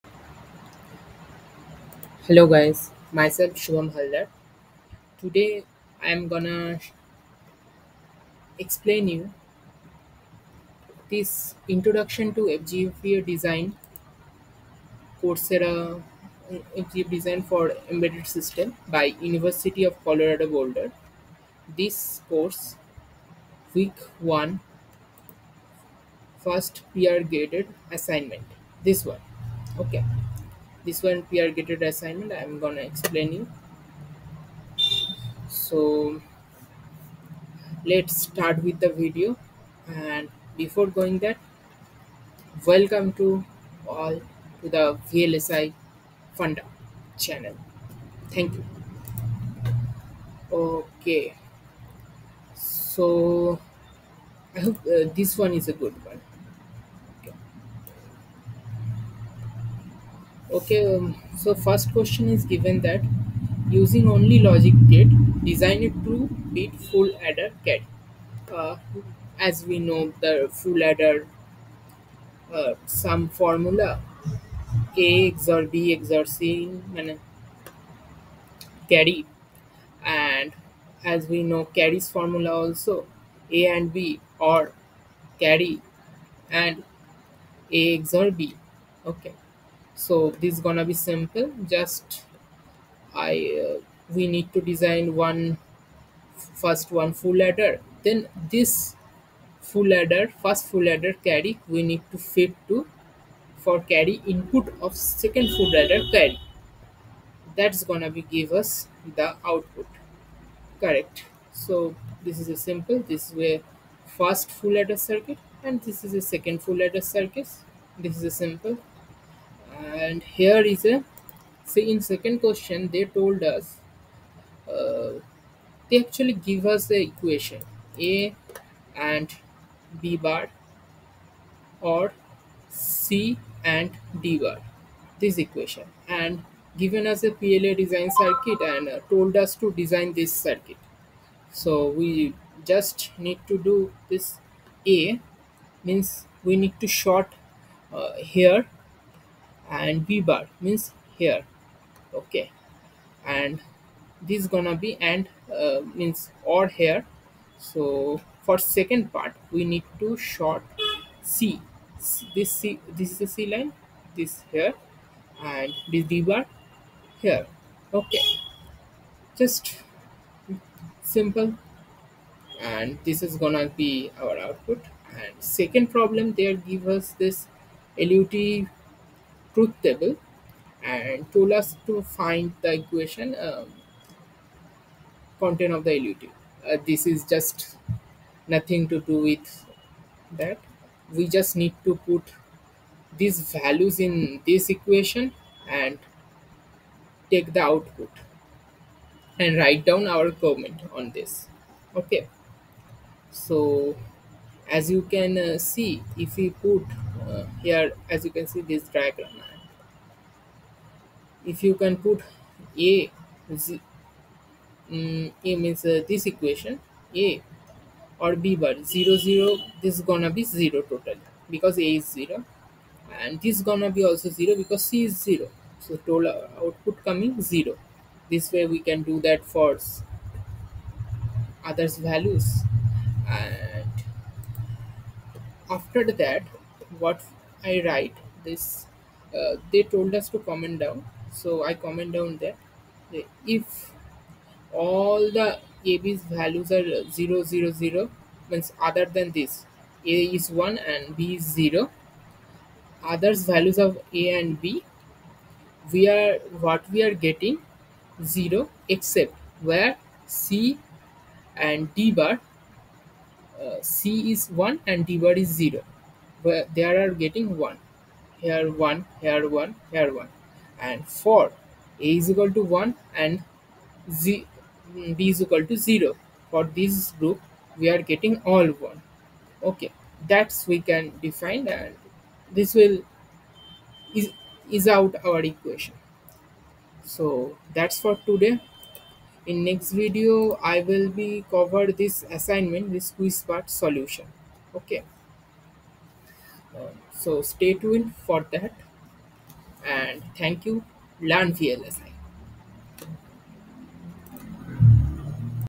Week 1 Application Assignment Solution | Introduction to FPGA Design for Embedded Systems | Coursera
Welcome to the Week 1 Application Assignment Solution video for the course "Introduction to FPGA Design for Embedded Systems" course of Coursera University of Colorado Boulder. In this video, I will guide you through the solution to the Week 1 assignment, helping you to solve the assignment smoothly.